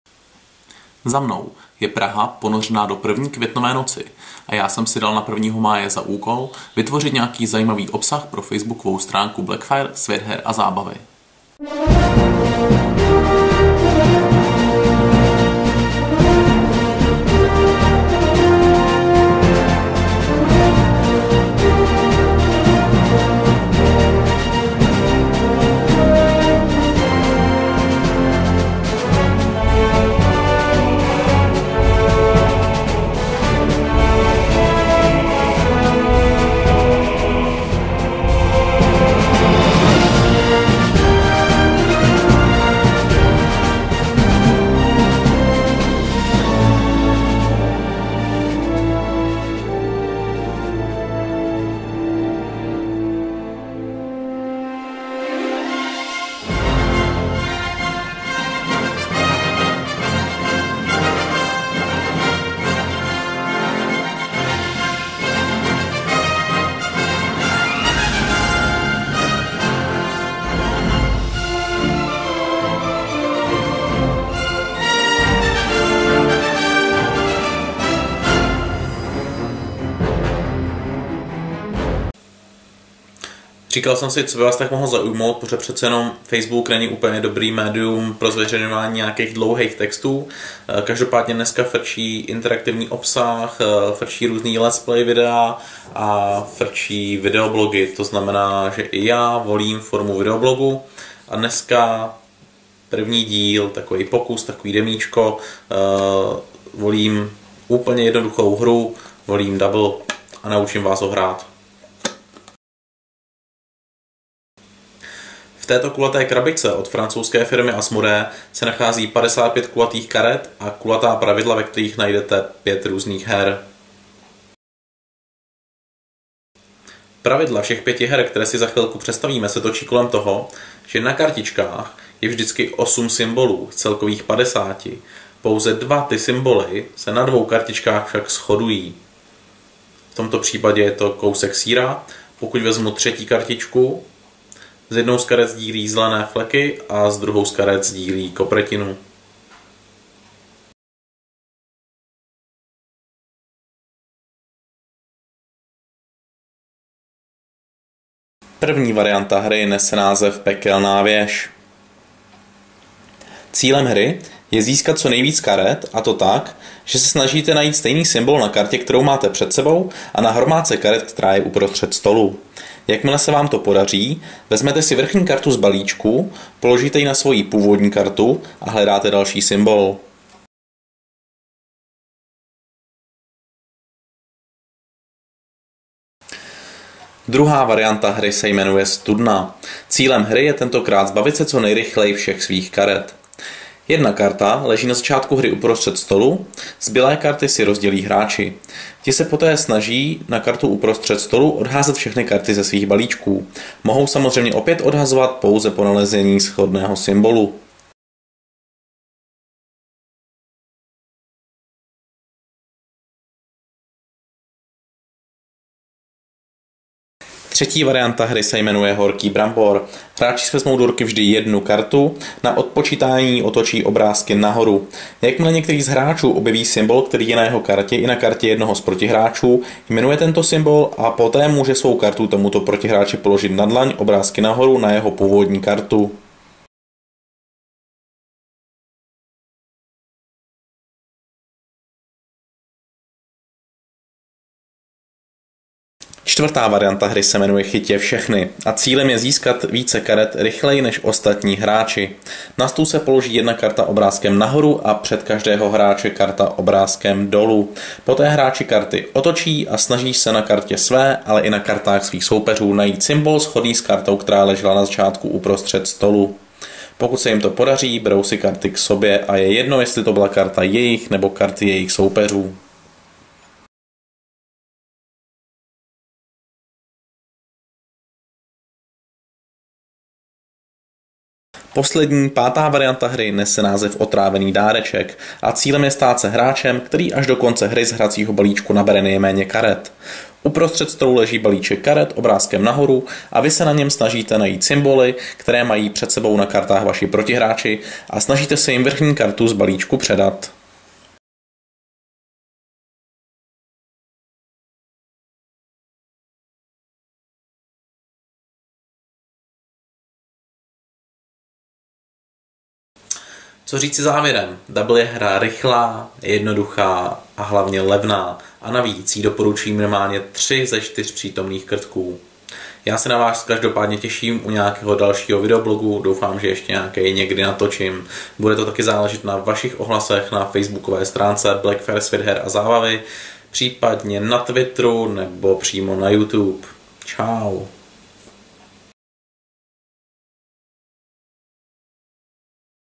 Krtkův herní video blog - 01 - Dobble
testovací díl video blogu pro facebookovou stránku Blackfire - Svět her a zábavy